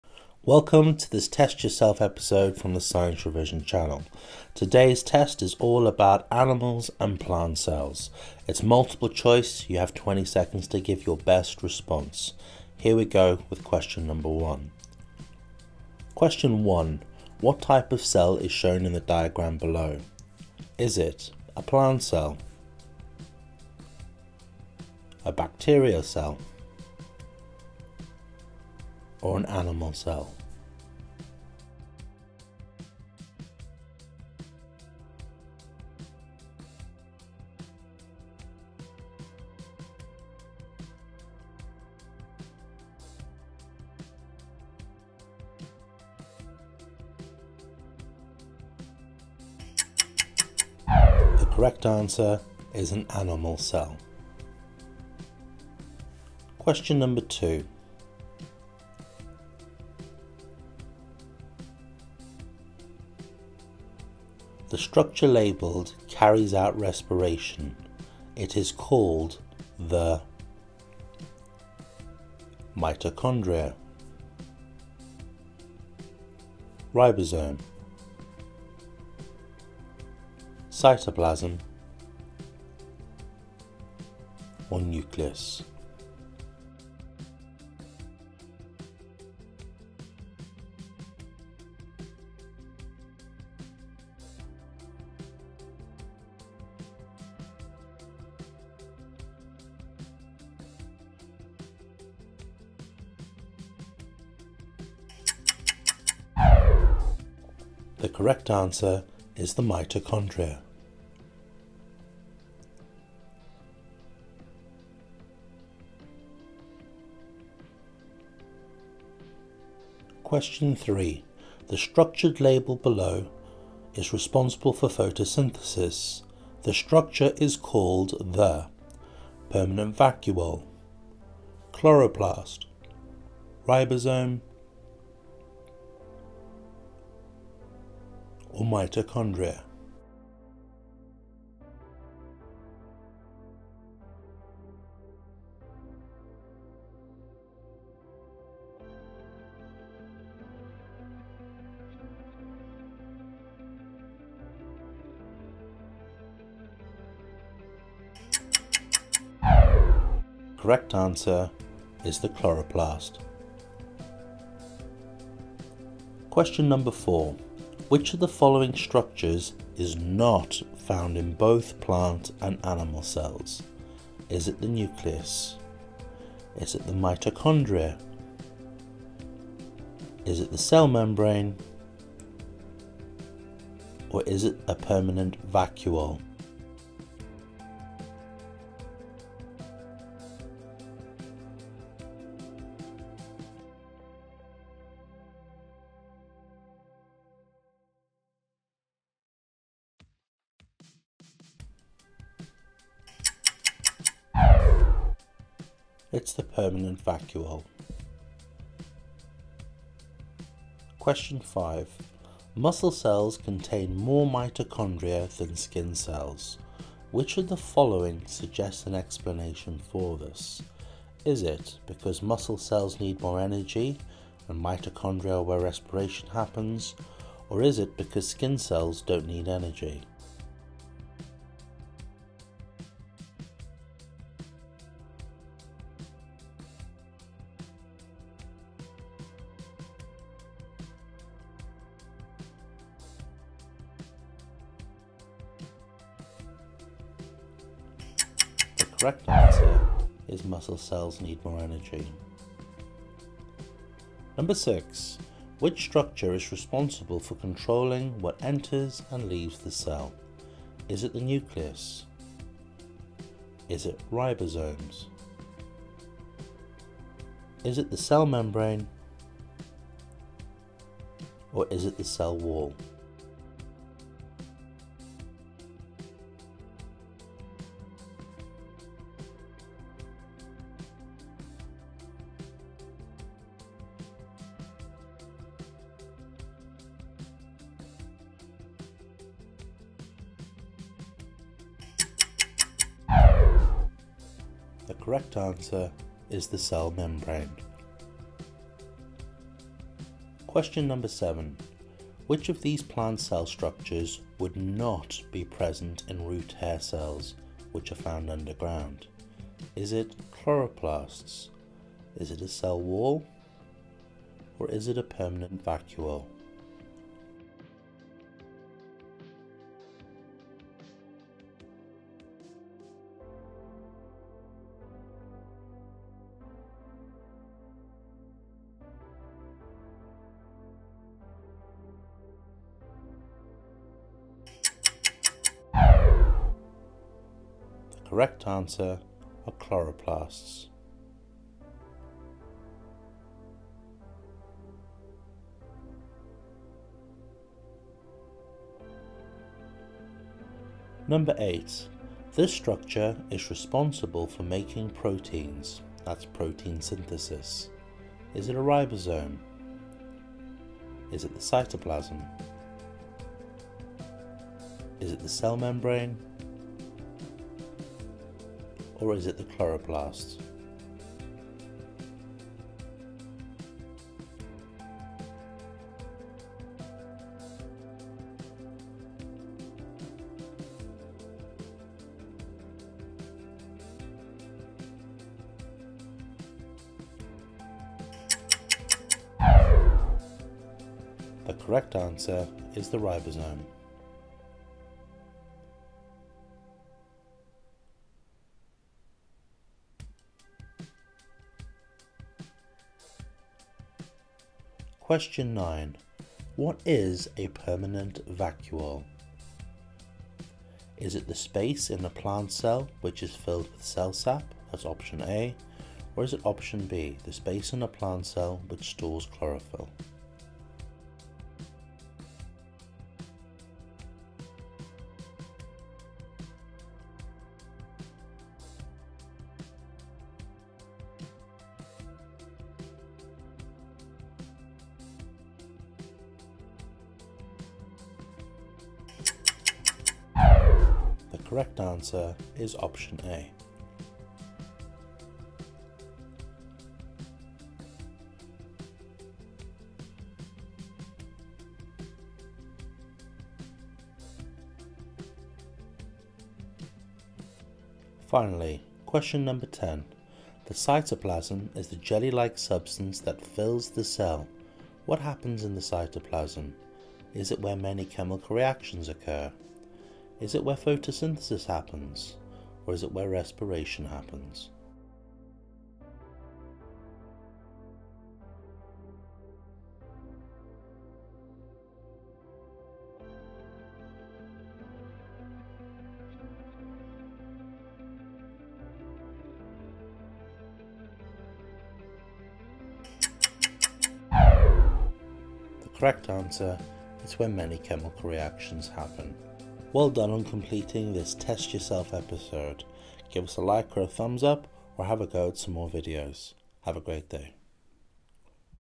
GCSE Science Biology - Animal and Plant Cells - Test Yourself
Test your knowledge on GCSE science biology animal and plant cells. This is core knowledge for biology GCSE and combined science 9-1 for paper 1. Questions cover the substructures of plant and animal cells and their functions to help you prepare for your GCSE exams.

All images by the Science Revision Channel
B1 Cell biology
Biology Paper 1
Combined Science Paper 1
AQA, OCR, IGCE and Edexcel
Science Revision